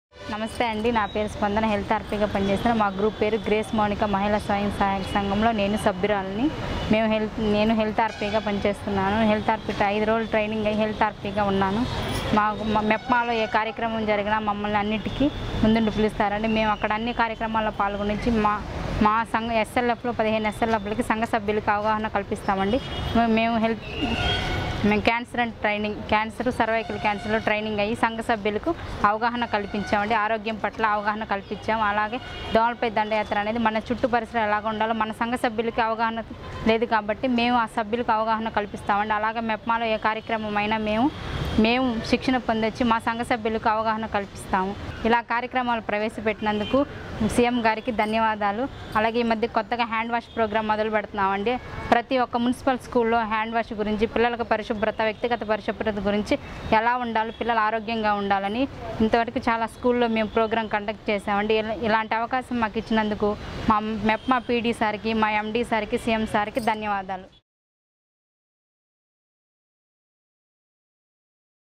Cancer Awareness through Health RPs | Srikakulam District
Government of Andhra Pradesh with the help of MEPMA is creating awareness amongst women about cancer through health resource persons.